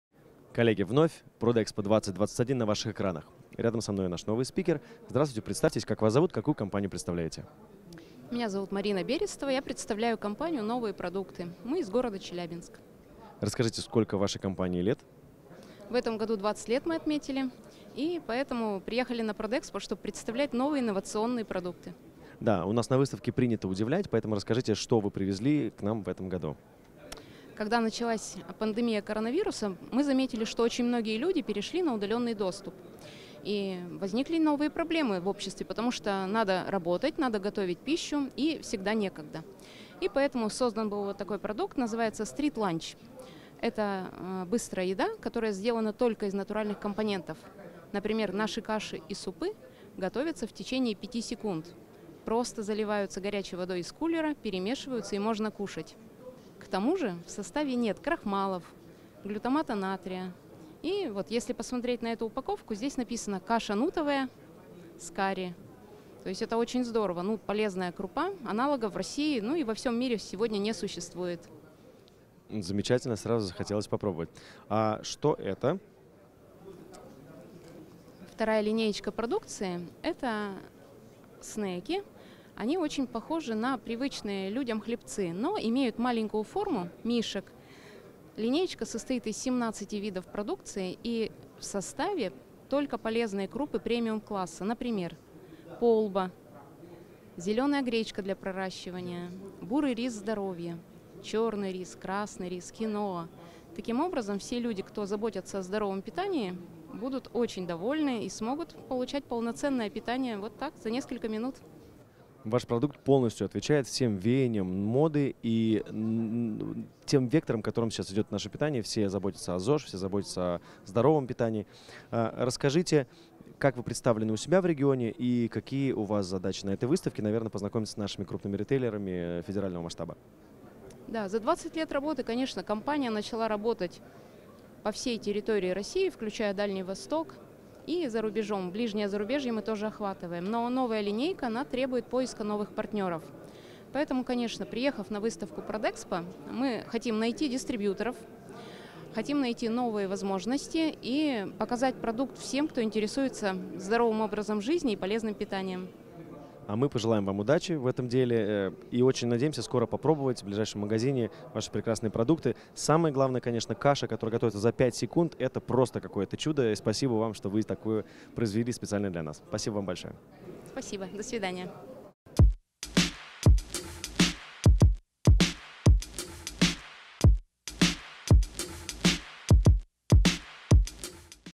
Марина Берестова - "Новые продукты" на
Марина Берестова, коммерческий директор ООО «Новые продукты», о быстрой еде из натуральных компонентов, а также снеках из полбы и зеленой гречки